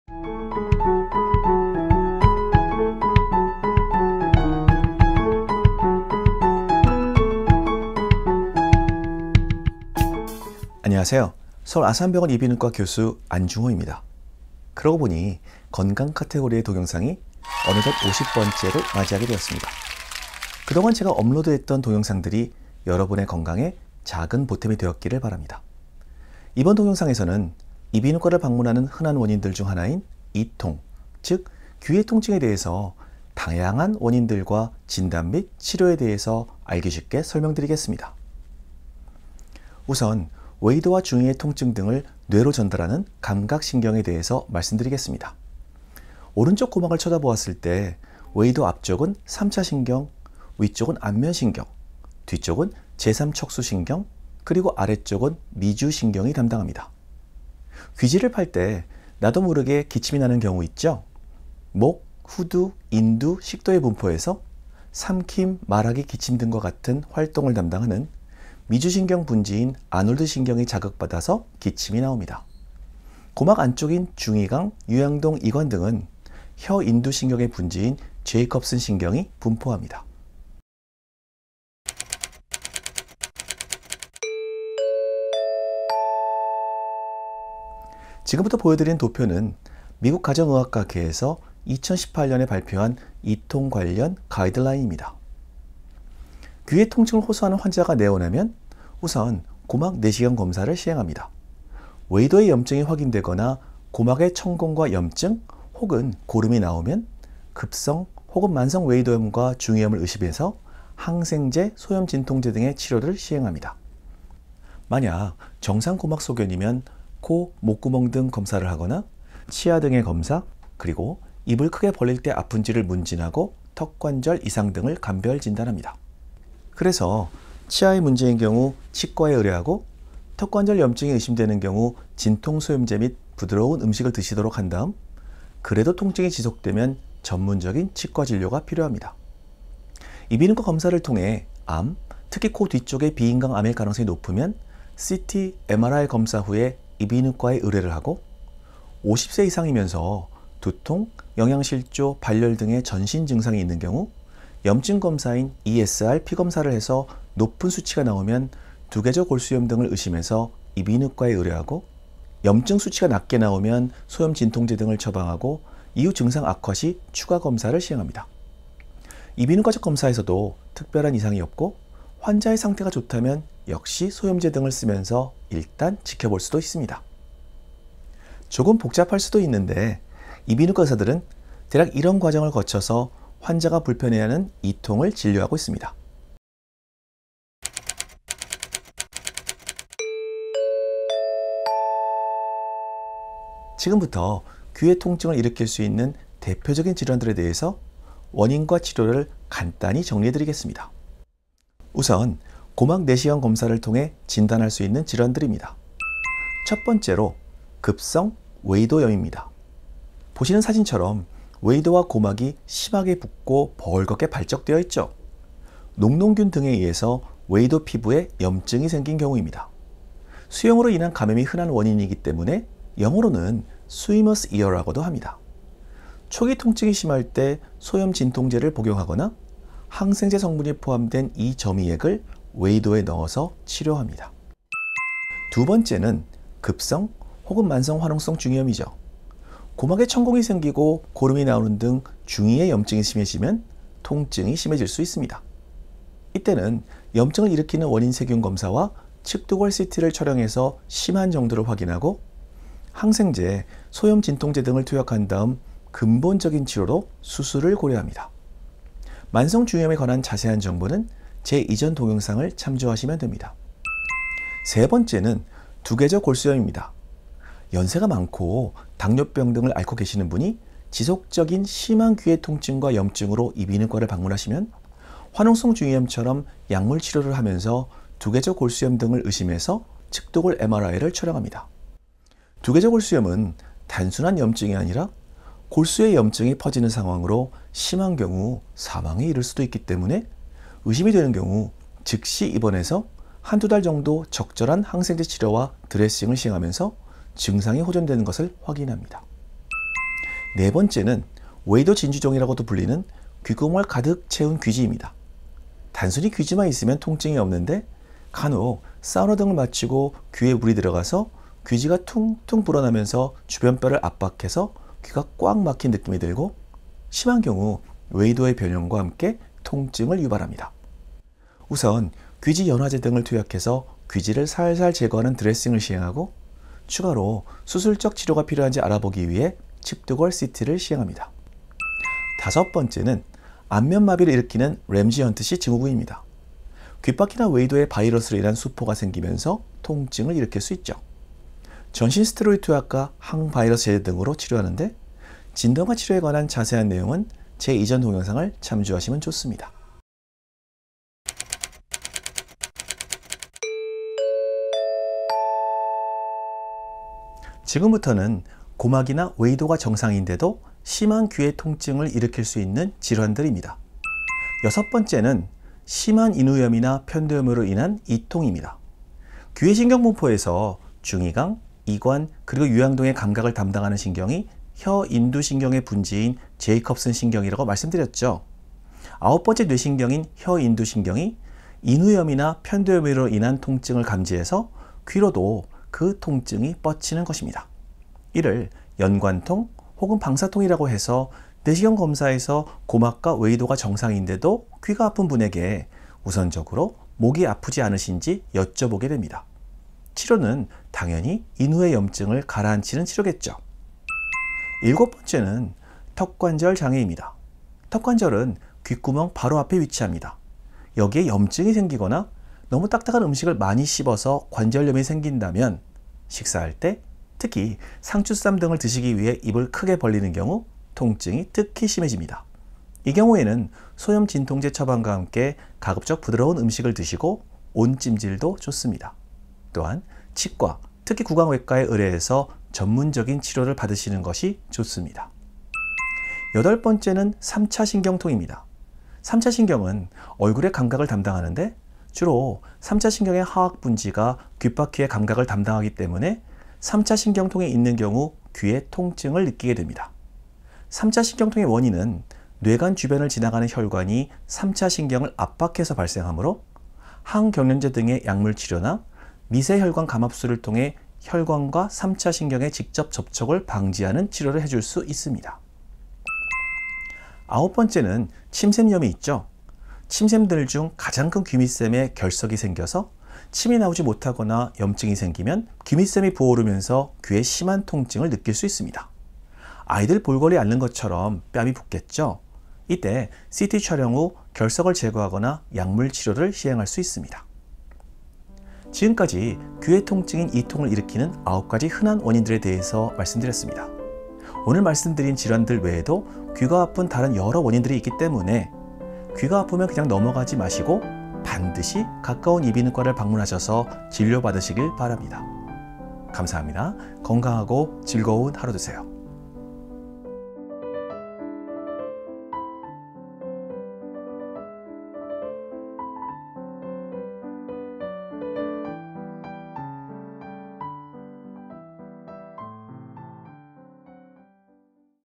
(건강) 귀의 통증이 발생하는 원인과 치료를 알려드립니다.
안녕하세요.
어느덧 이어드림 채널의 건강강좌 동영상이 50번째를 맞이하게 되었습니다. 그동안 제가 만들었던 영상들이 여러분의 건강에 작게나마 도움이 되었기를 바랍니다.

이번 동영상에서는 이비인후과를 방문하는 중요 원인들 중 하나인 귀의 통증, 이통에 대해서 가장 흔하게 접할 수 있는 9가지 질환(귀로 인한 질환 5가지, 귀 외의 질환 4가지)에 대해 간단히 설명드리겠습니다.

본 동영상이 여러분의 귀건강에 작은 도움이 되기를 바랍니다.

감사합니다. 건강하고 즐거운 하루 되세요.